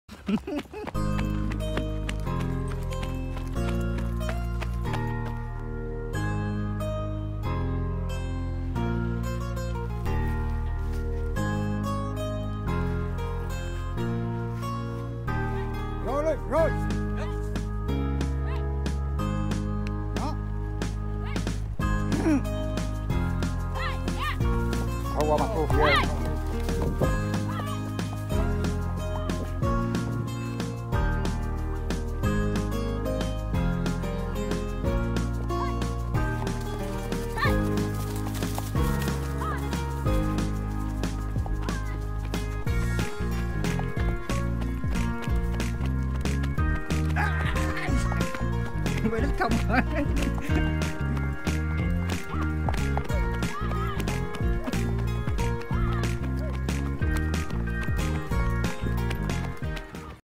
Yaya Trương Nhi- diễn Viên Quốc Cường " Lần đầu cưỡi ngựa"
Coi đi rồi biết, tập cho Film mới,không có dễ và sung sướng tí nào😱😱😱